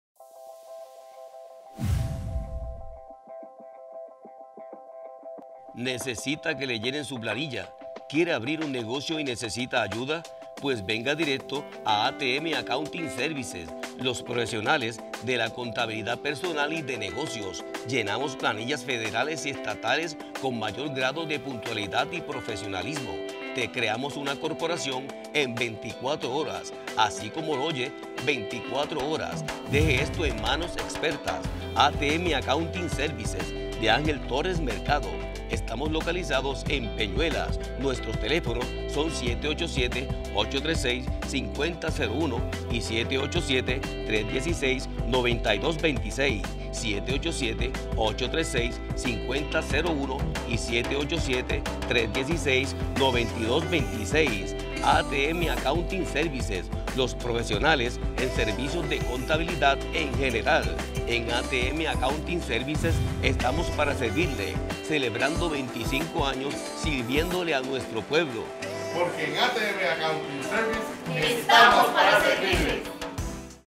ATM Accounting Services
En nombre de Antonio "Zanio" Alvarez y mis compañeros de Conoce a Peñuelas, queremos agradecer a ATM Accounting Services y todo su equipo de trabajo por su aportación y apoyo a la campaña "Hombres Reales Visten de Rosa"la cual este año 2018 me honra en ser parte. Esta campaña es a beneficio de la Sociedad Americana Contra El Cáncer de Puerto Rico, y todo lo recaudado en la misma será100% dirigido a dicha causa. La cantidad total recaudada será anunciada en el cierre de " Marcha Por Una Causa: Cáncer de Seno" de nuestro pueblo de Peñuelas el próximo 19 de octubre.
Una vez más gracias por ayudarme a ayudar.